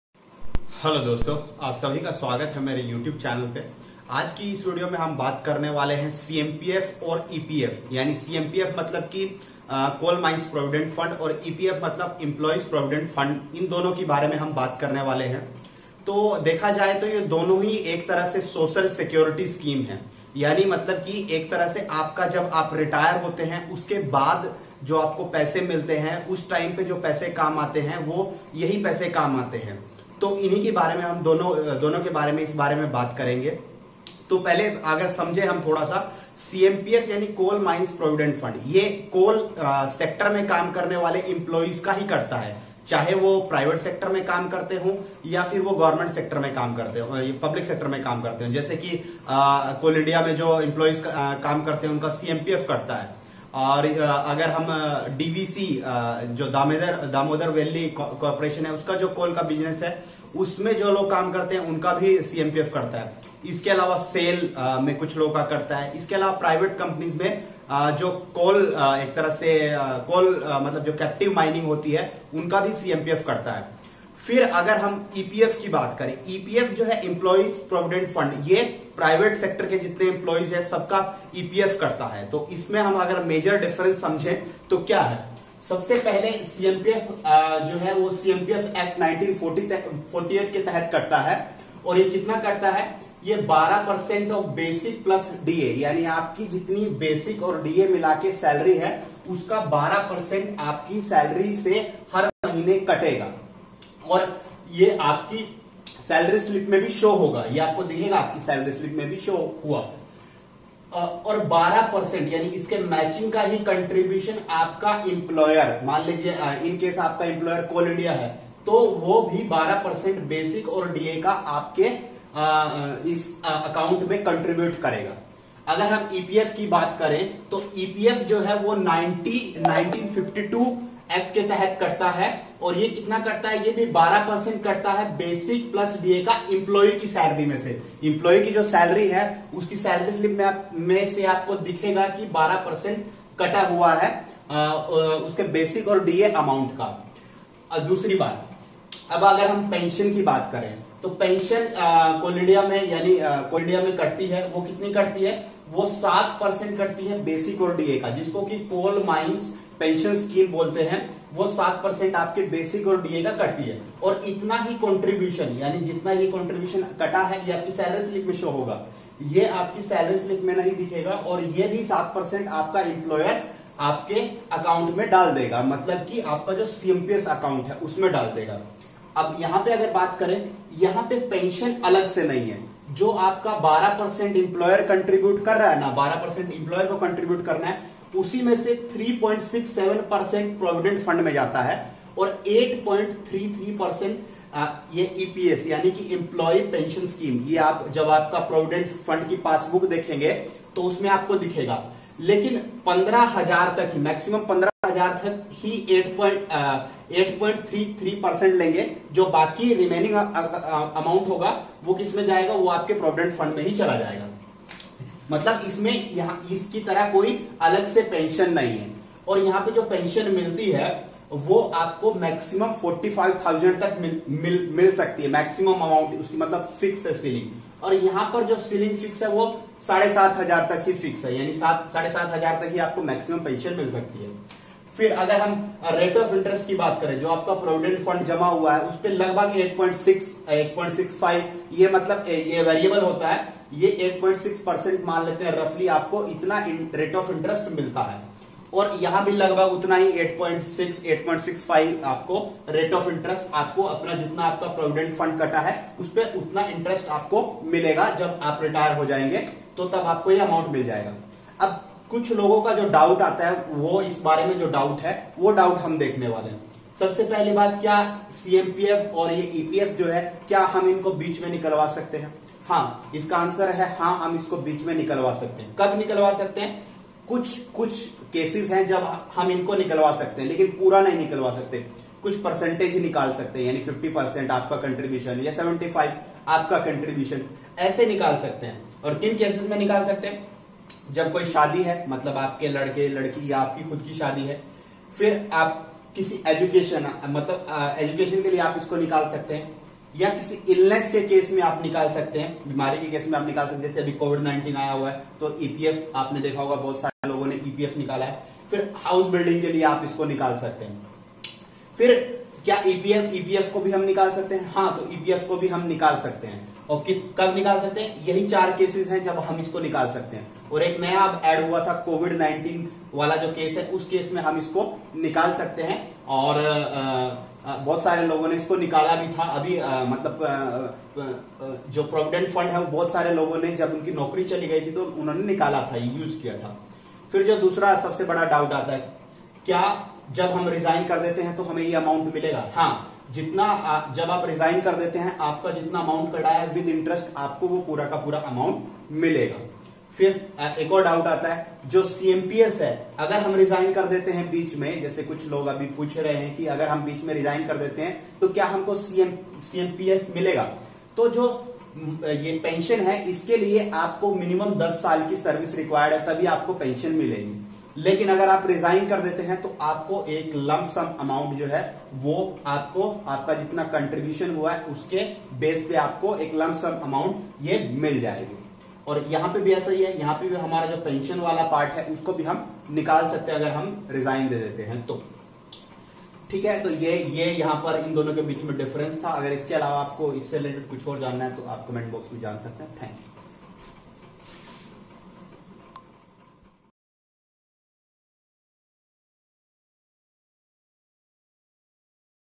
CMPF( Coal Mines Provident Fund) & EPF( Employee Provident Fund) Explained
I will discuss about two social security scheme in this video